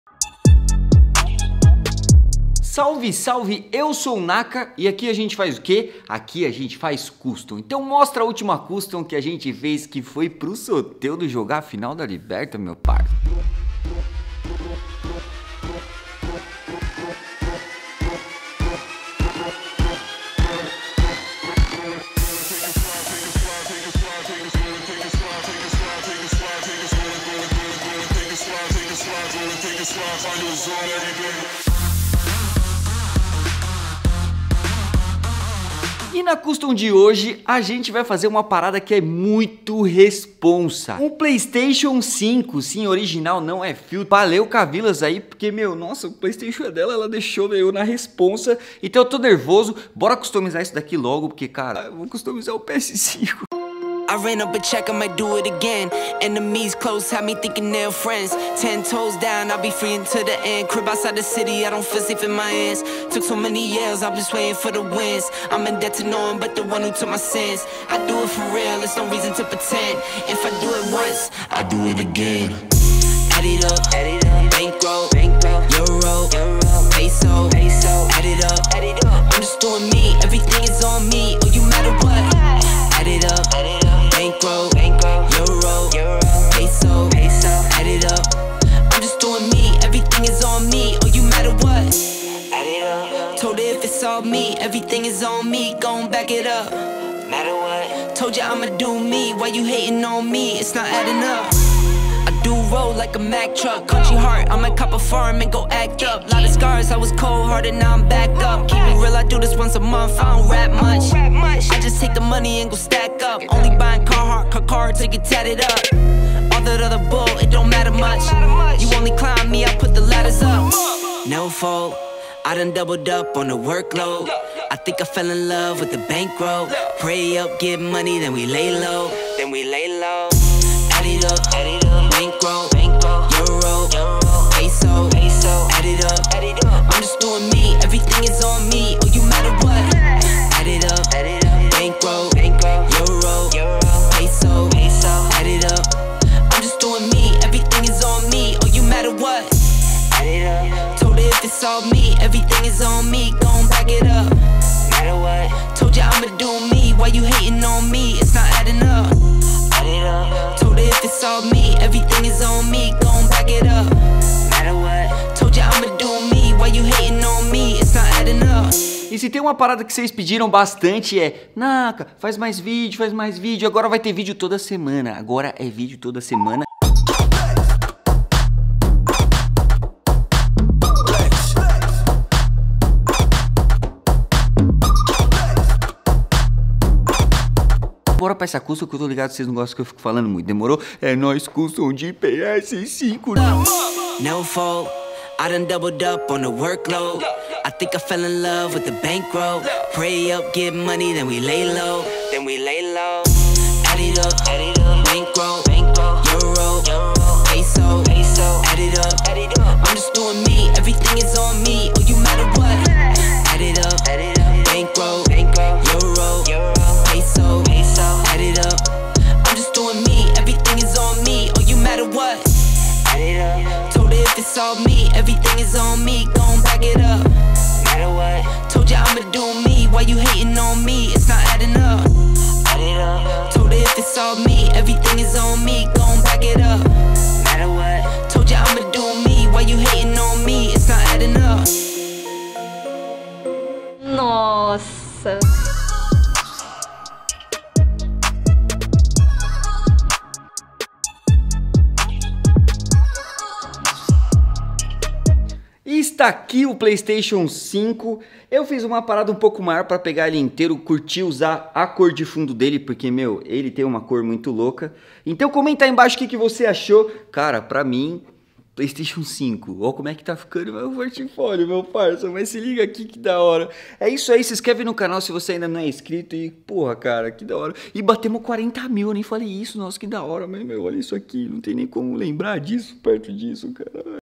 CUSTOMIZEI UM PS5 🎮
Customizei um PS5 para minha amiga e achei ZIKA!